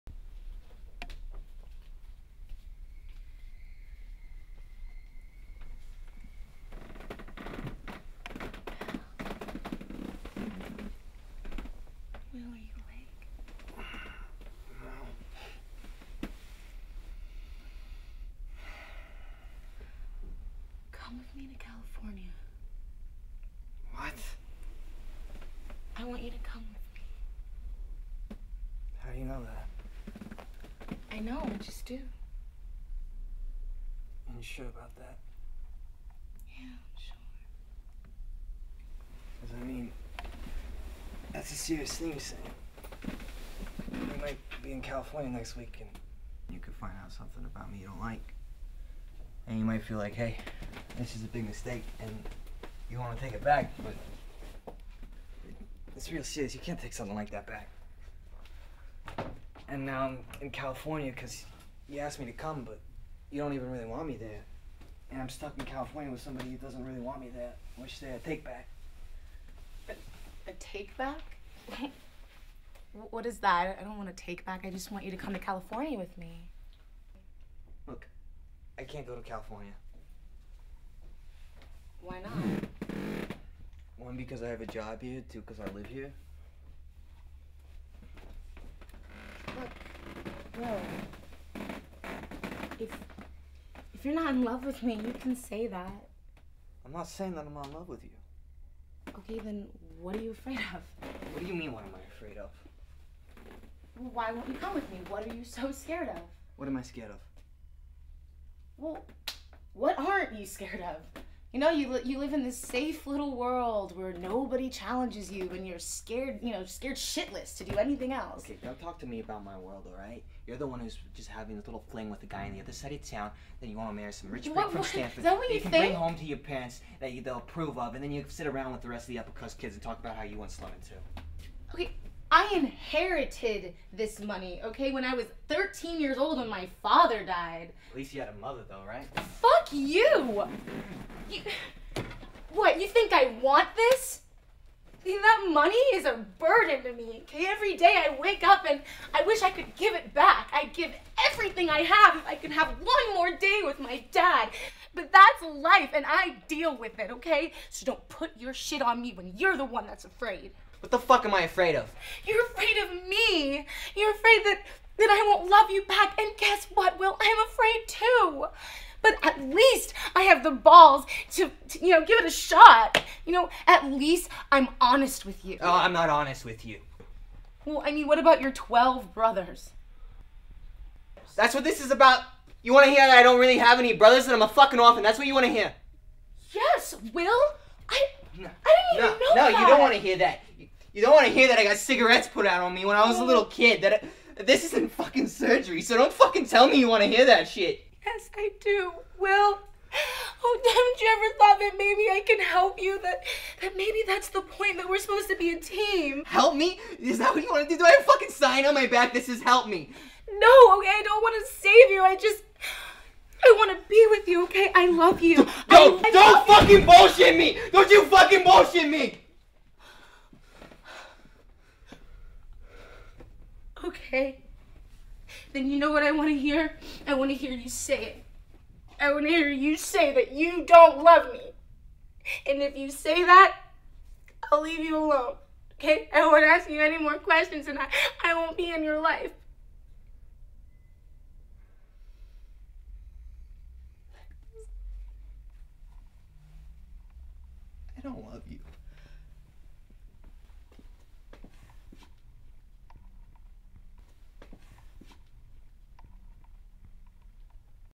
"GOOD WILL HUNTING" directing workshop
A scene from "Good Will Hunting" I directed for class. Let me know what you think.